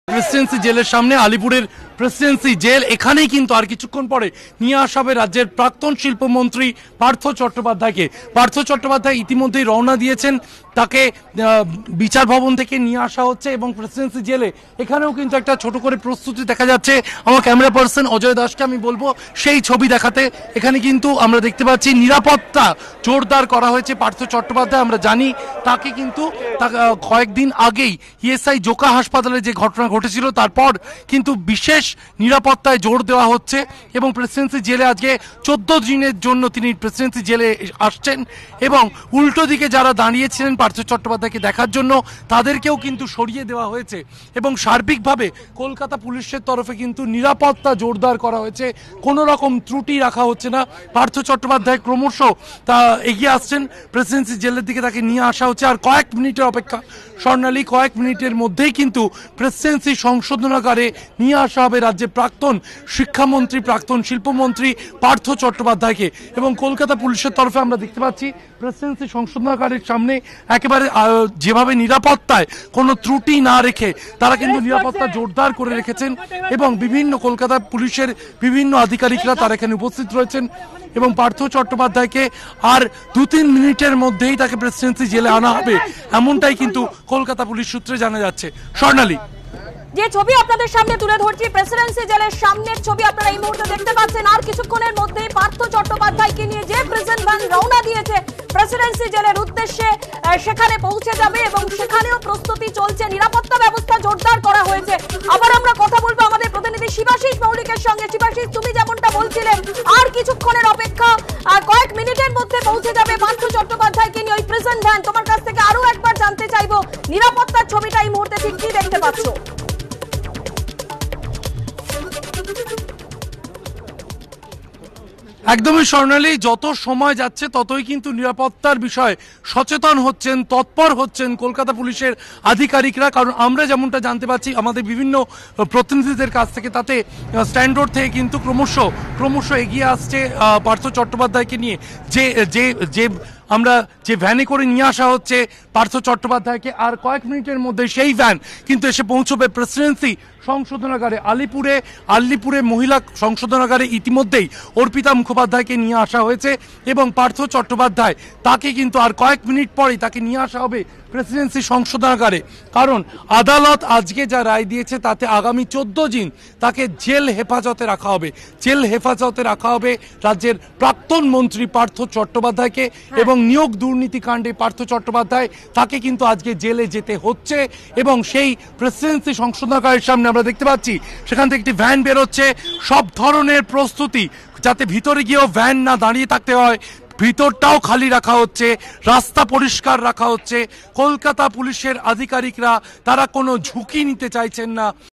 SSC Scam: ১৮ অগাস্ট পর্যন্ত জেল হেফাজত পার্থ-অর্পিতার, জেলেও চলতে পারে জেরা
পার্থ-অর্পিতাকে জেল হেফাজতের নির্দেশ ব্যাঙ্কশাল কোর্টের। ১৪ দিনের জেল হেফাজতের নির্দেশ ব্যাঙ্কশাল কোর্টের
পার্থর জামিনের আবেদন খারিজ ব্যাঙ্কশাল কোর্টের। ১৮ অগাস্ট পর্যন্ত পার্থ-অর্পিতার জেল হেফাজত। জেলে গিয়ে পার্থকে জেরা করতে পারে ইডির ২ আধিকারিক।